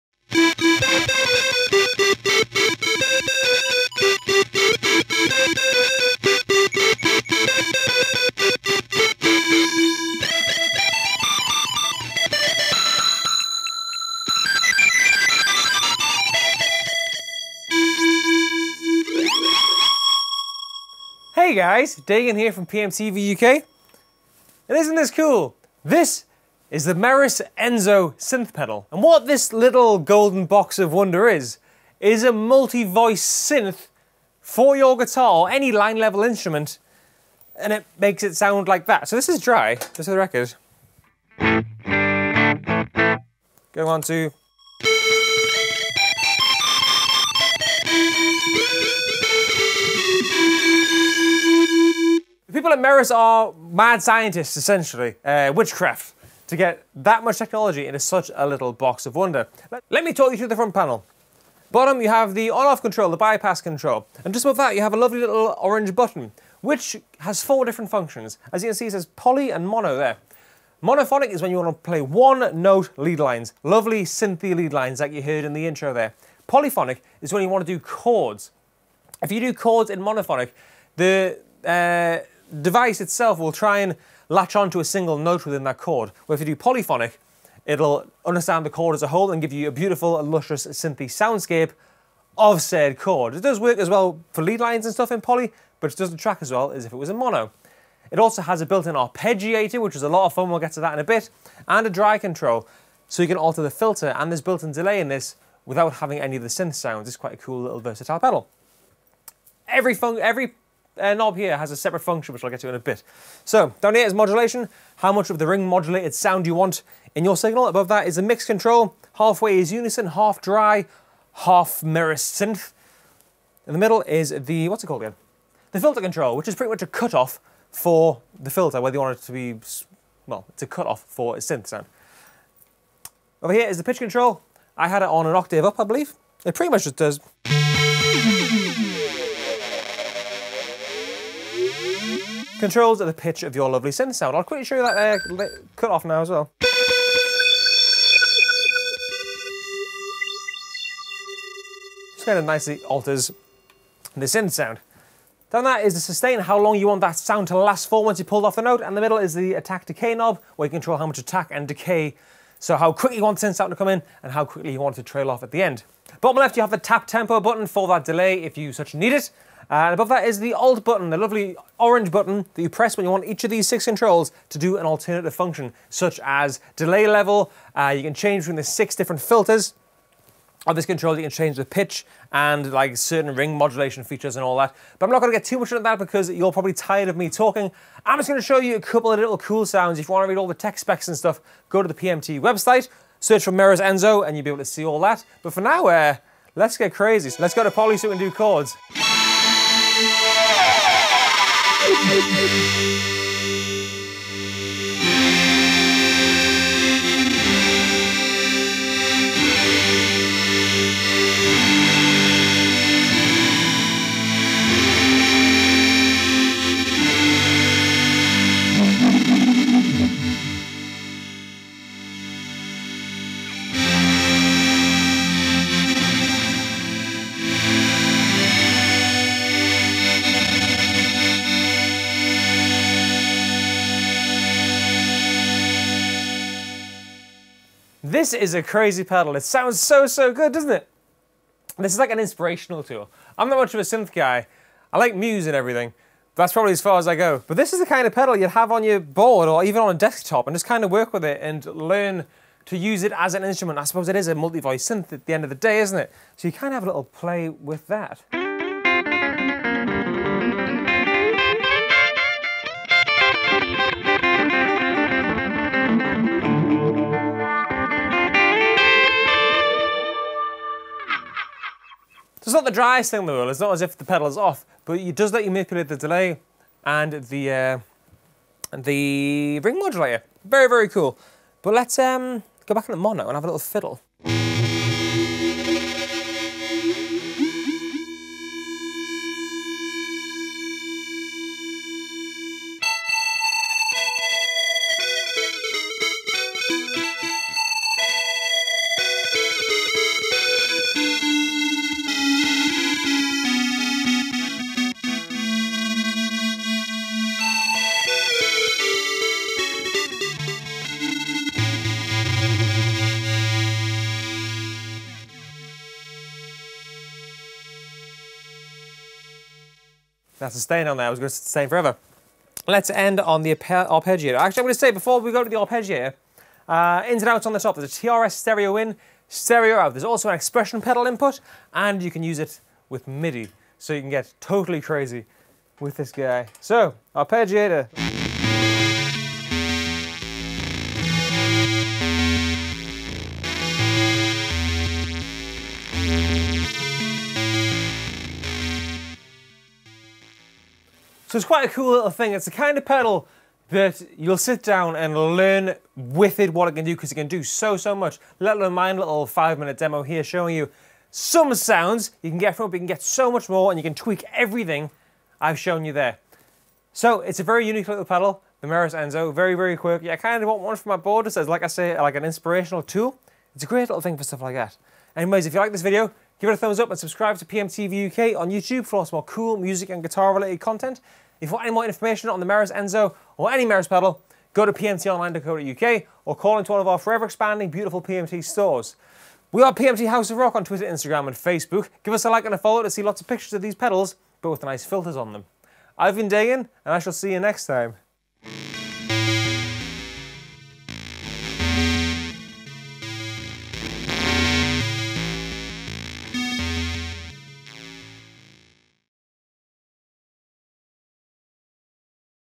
Meris Enzo Synth Pedal Review - The Most Powerful Synth Pedal Ever?
Dagan reviews the Meris Enzo Synthesizer Pedal with the all new Gibson SG 61 Original.

If you want to completely blow peoples minds with your sound, then the boutique multi-voice Meris Enzo Synth Pedal is a must have. This is one of the best synthesizer pedals you can get your hands on especially if you want to recreate the sounds of iconic 80s films and synthesizer music with your guitar.

This is not your regular guitar synthesizer pedal as it features a wide range of amazing aspects such as Dual Oscillator Synth architecture with selectable waveforms. There’s also a two tap delay with Feedback, Modulation, and special two stage level control. The addition of two distinct envelope types such as Triggered and Envelope and three synth modes: Poly Synth, Mono Synth, and Arpeggiated Synth – there’s a world of different sounds at your disposal!